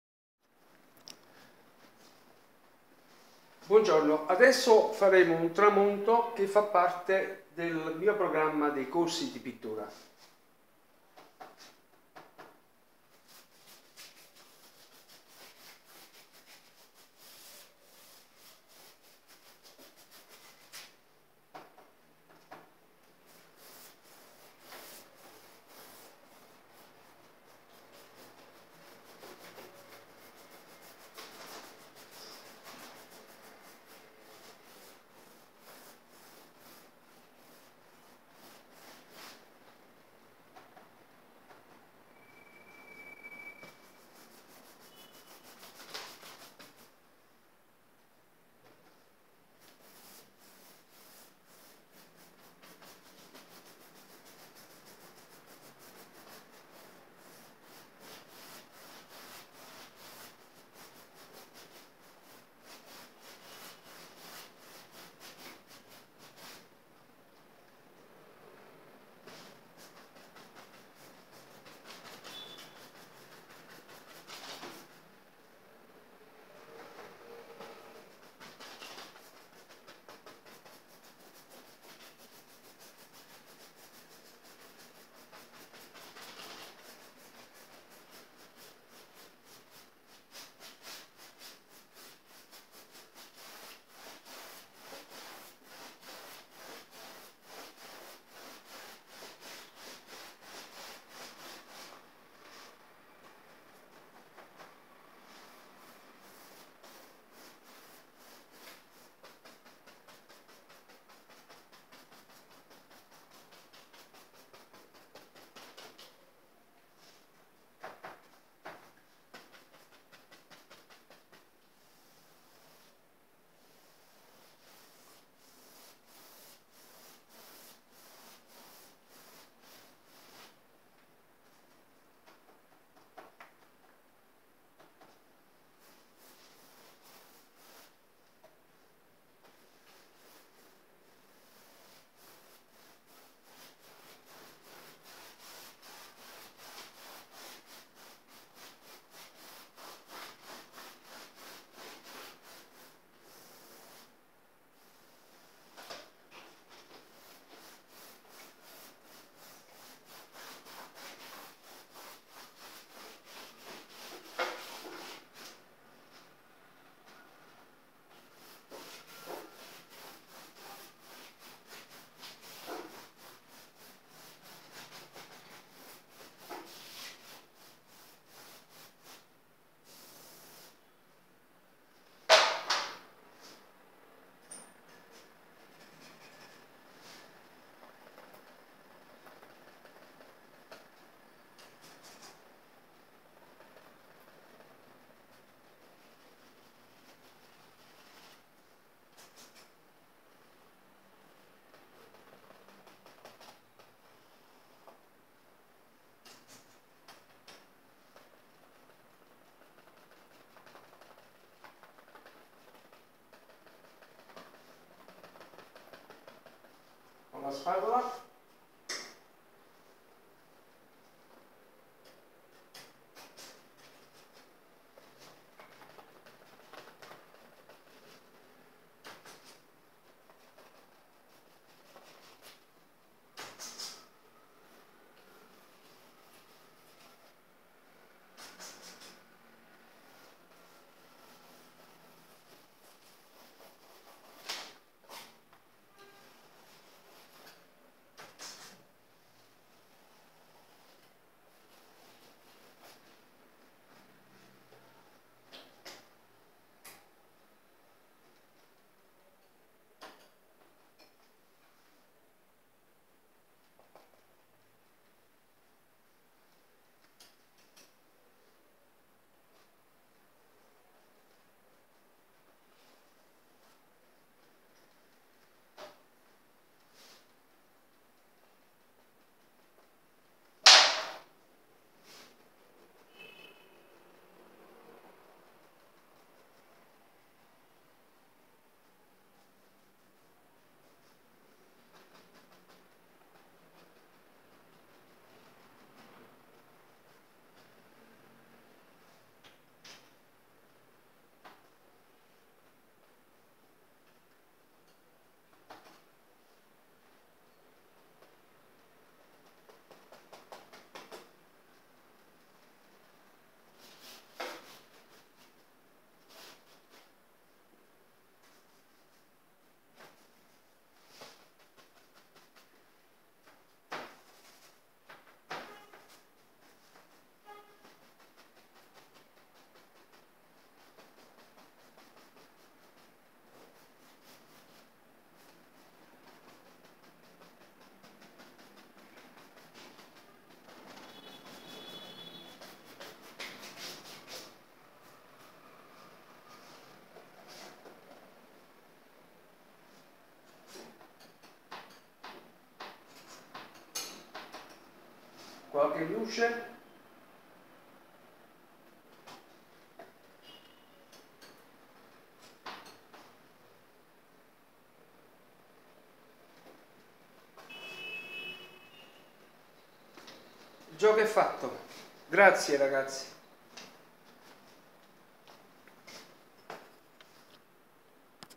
Come dipingere un tramonto in pochi minuti
Ho dipinto in 6 minuti questo video integro e con velocità normale facendo vedere ai miei allievi e agli appassionati di arte le tecniche veloci che insegno ai miei corsi d"arte sempre però con basilari nozioni accademiche, ma
 molto semplicate.
La bottega dell'Arte  Pino Alfieri artista pittore Facebook.